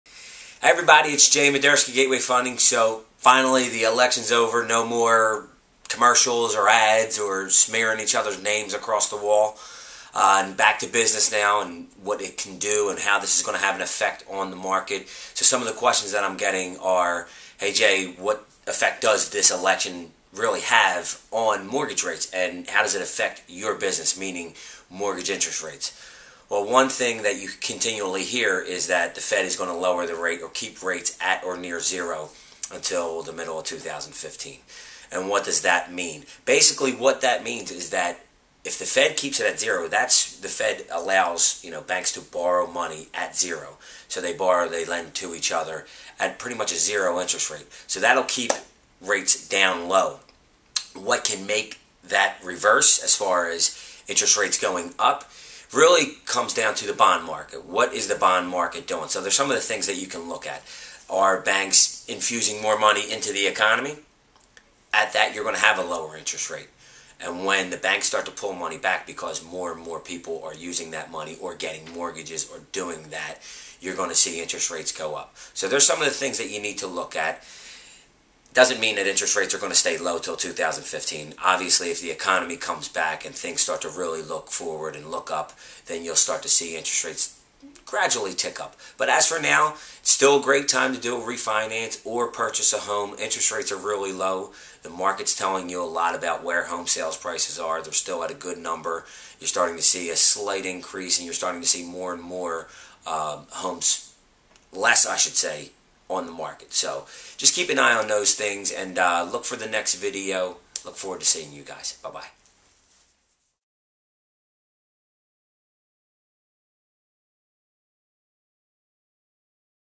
Jay Moderski - Post Election Mortgage Update
So the election is over and all the homeowners and homebuyers that had sat back and waited to see what might happen can now let out their collective breath and move on. There are some things to be aware of moving forward however, so that you can maximize your opportunities as a buyer (and seller). Watch this week's video blog to find out more on the post-election mortgage update.

Jay Moderski
Branch Manager and
Mortgage Originator
NMLS #: 133515